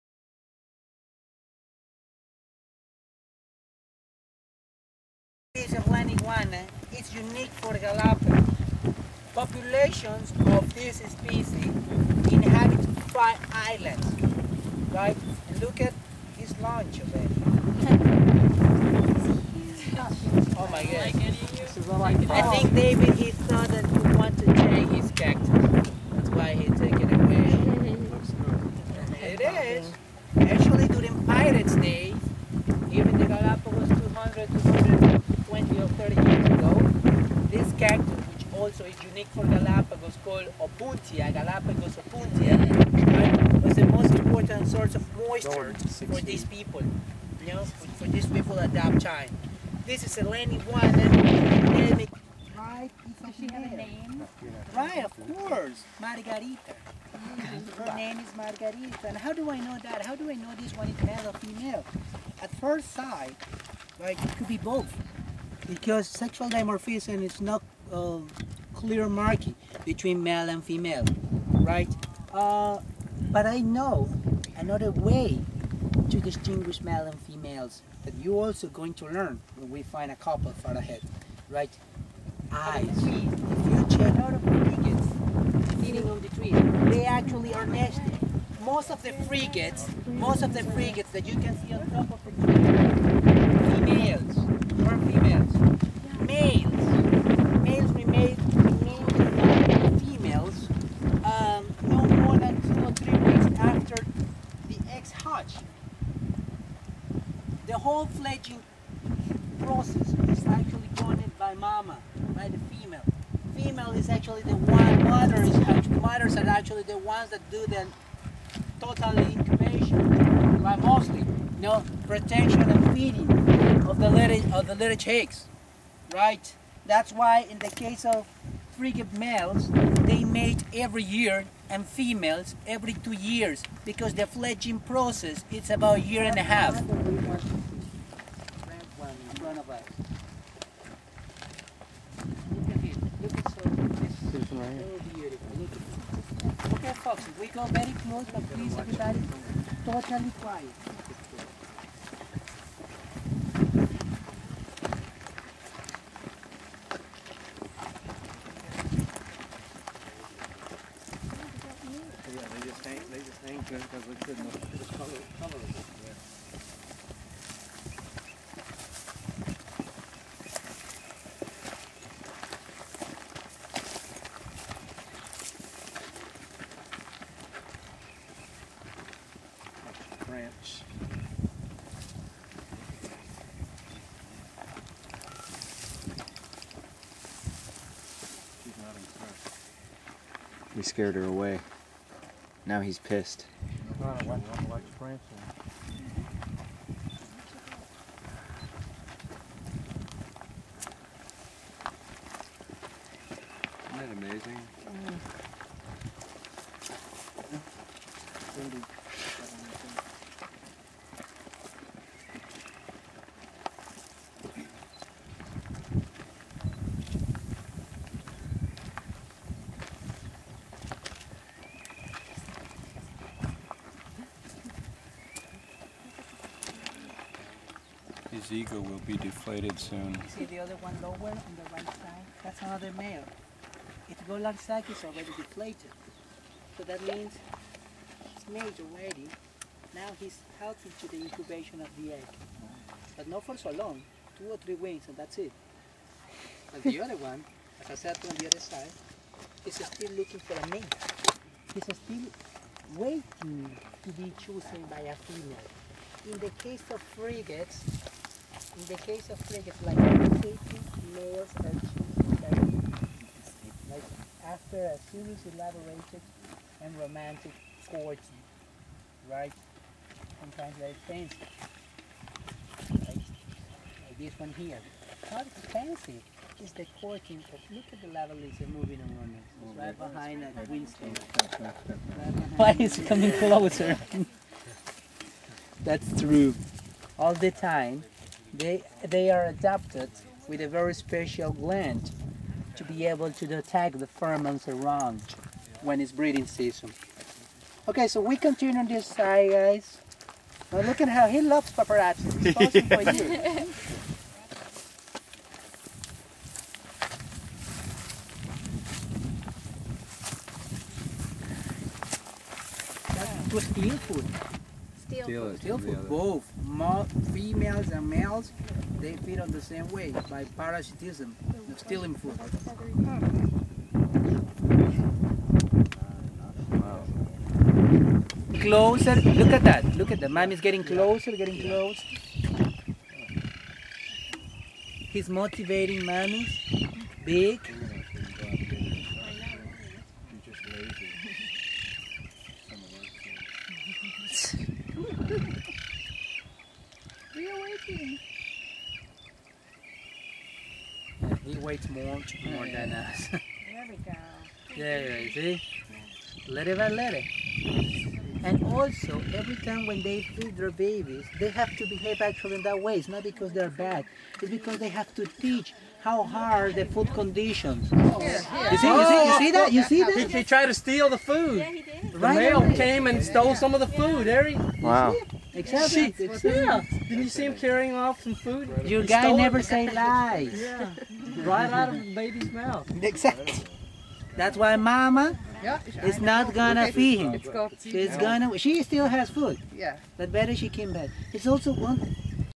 Galapagos Islands: North Seymour Island
Frigates and Land Iguanas!  Frigate thiefs!!